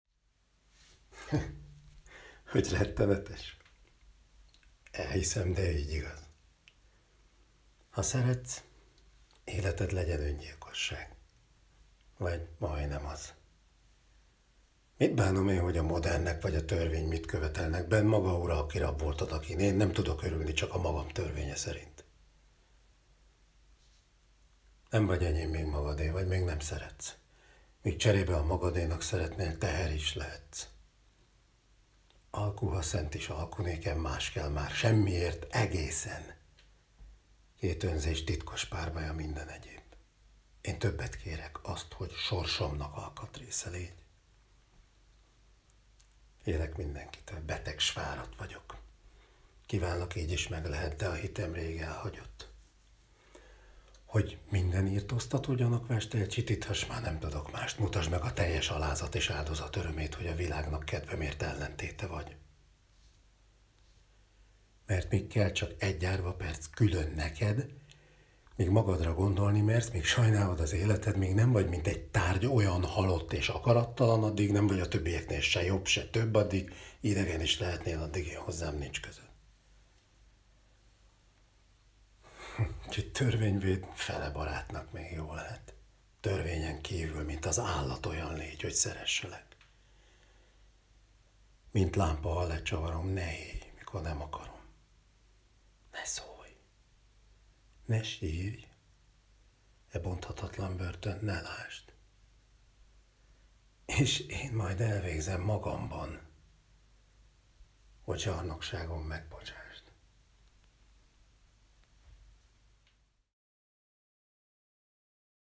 Szabó Lőrinc: Semmiért egészen
Elmondja: Gosztonyi Csaba